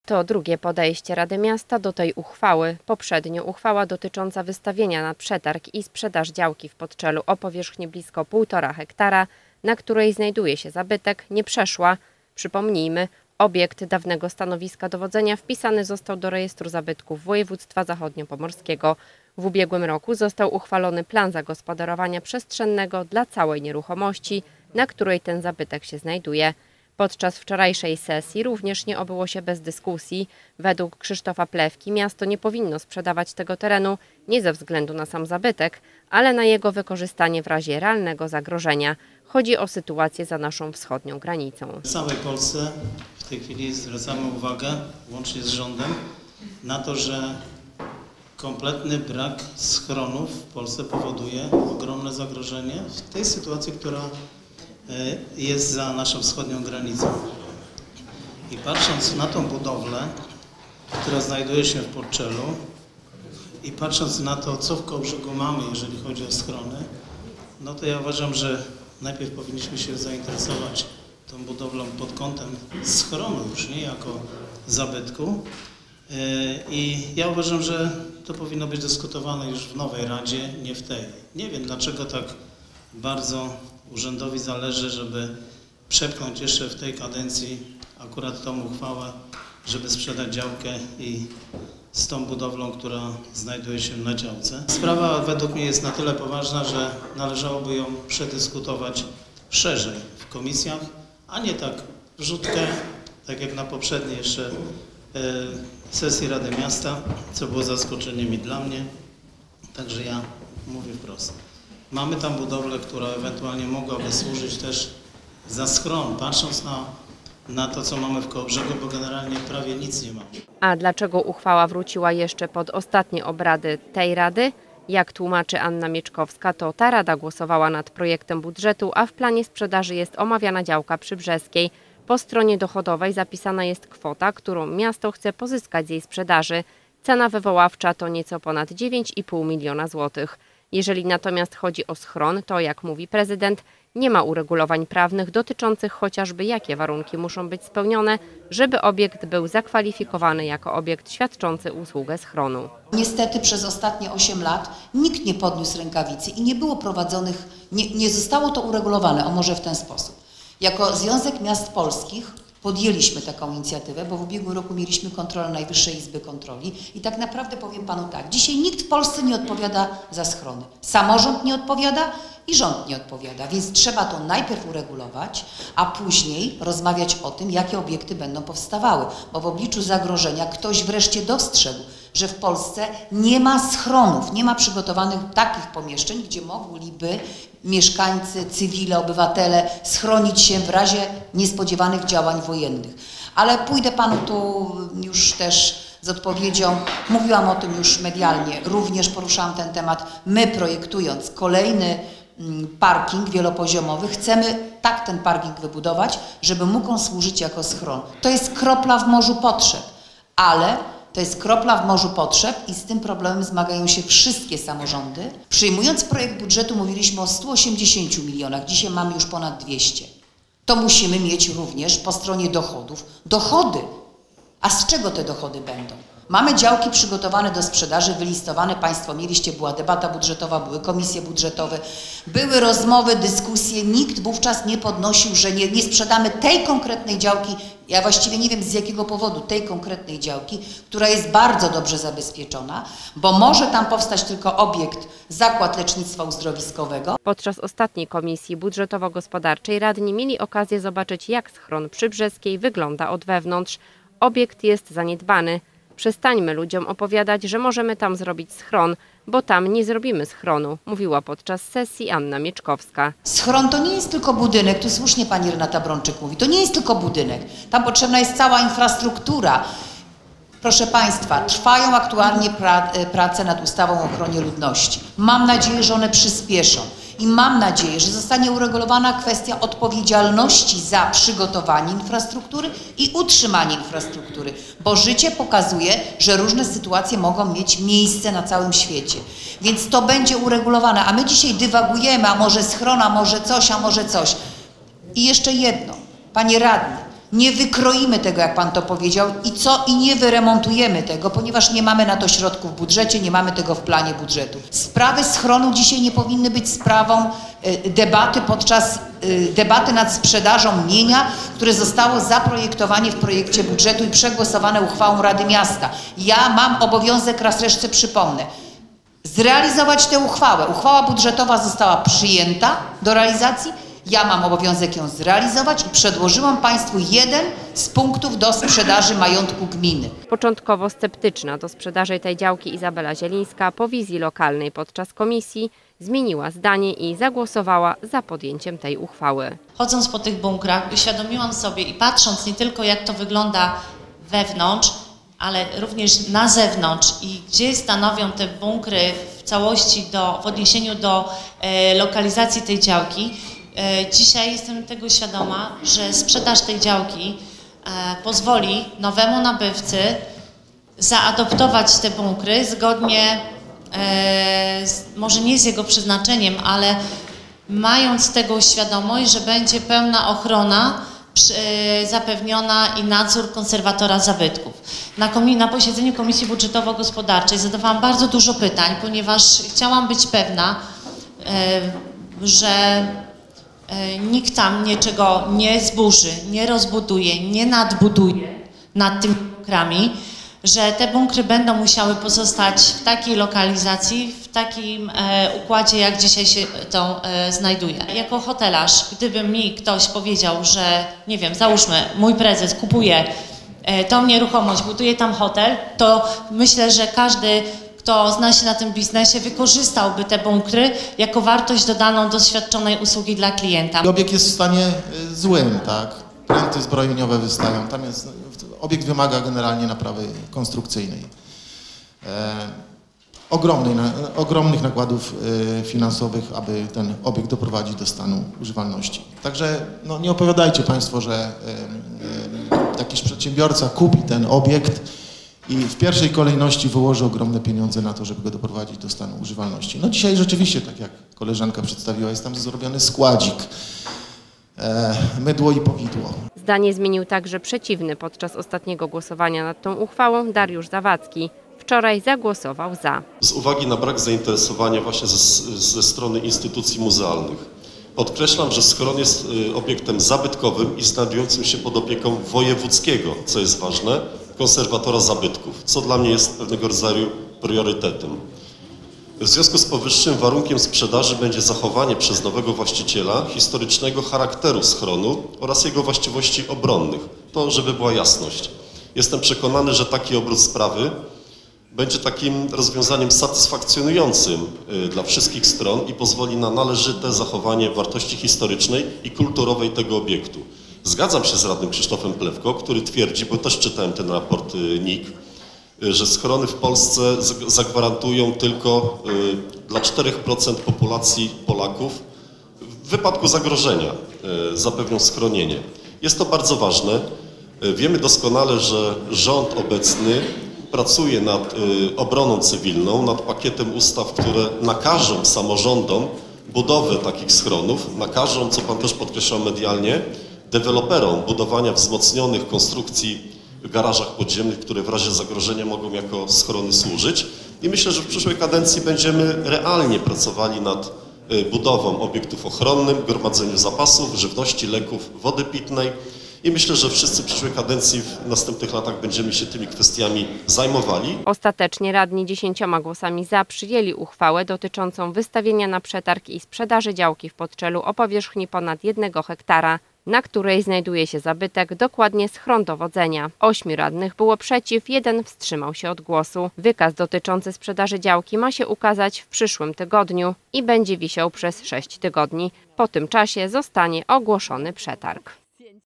DZIAŁKA PRZY ULICY BRZESKIEJ NA SPRZEDAŻ
Podczas wczorajszej sesji radni podjęli uchwałę dotyczącą wystawienia na przetarg i sprzedaży działki w Podczelu o powierzchni ponad 1 hektara, na której znajduje się zabytek, dokładnie schron dowodzenia.